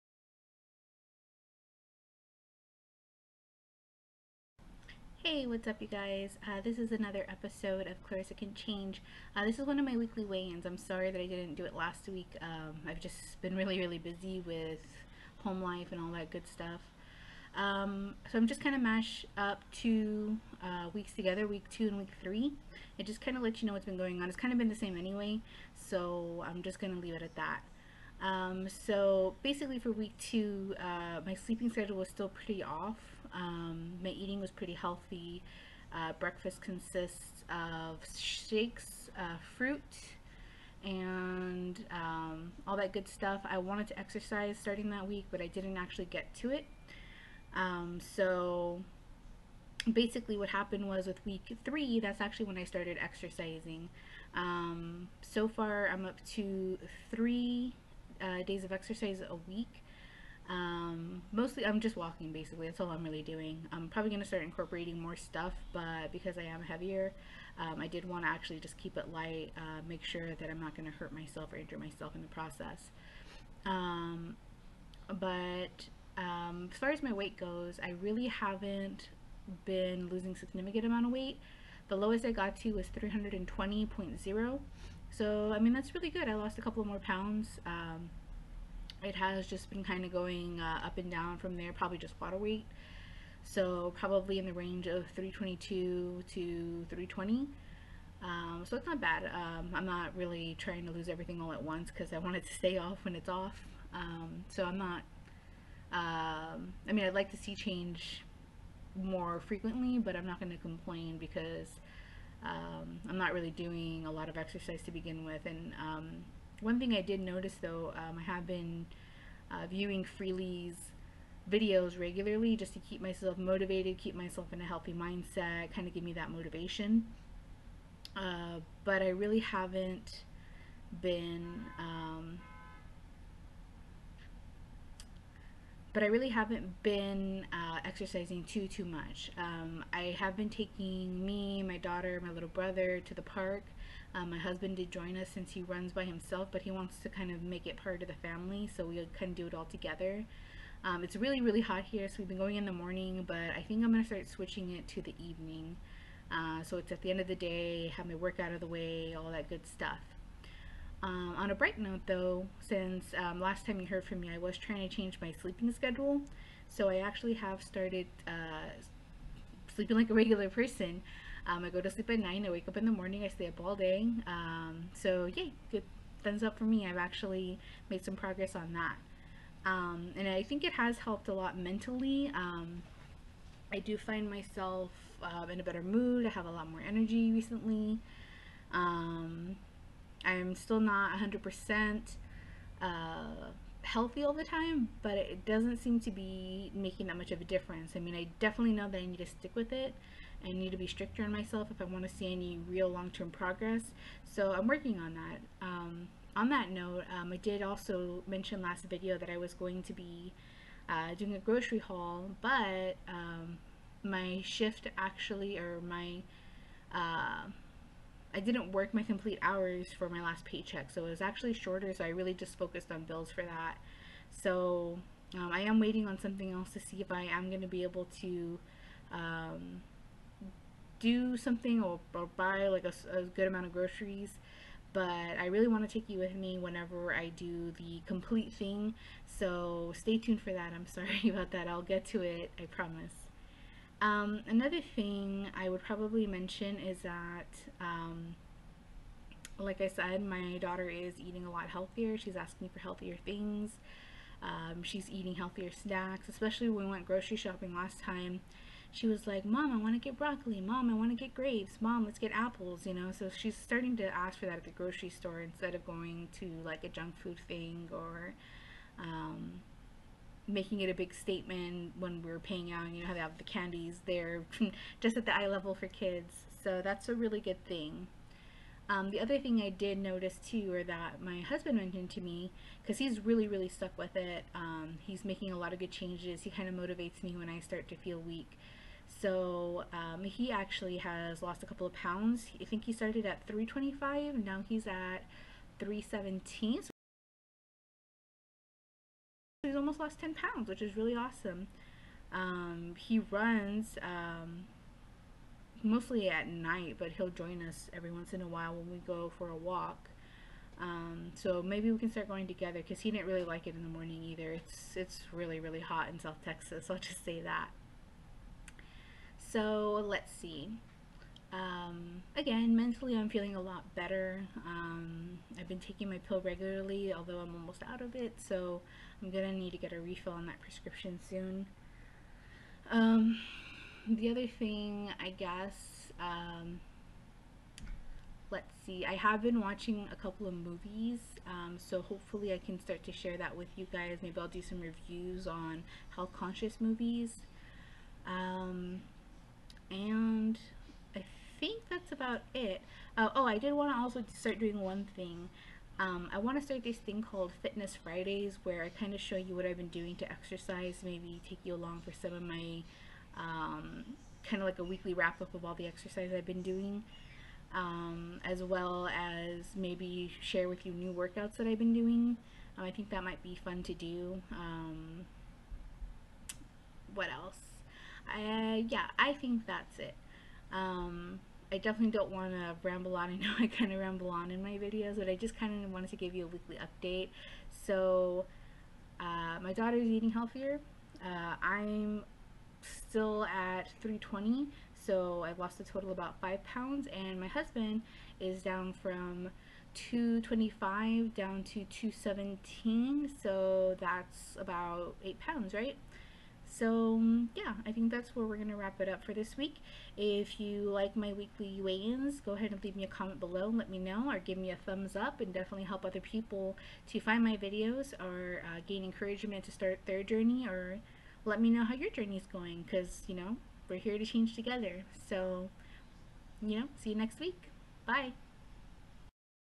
Weekly Weigh In: Weeks 2 & 3 - MY SLEEPING PATTERNS + I'M EXERCISING NOW! YAY!
Catch up on my weekly weight in and catch up on the past two weeks.

I talk about my new sleeping habits, how I started exercising, progress on my weight loss, etc.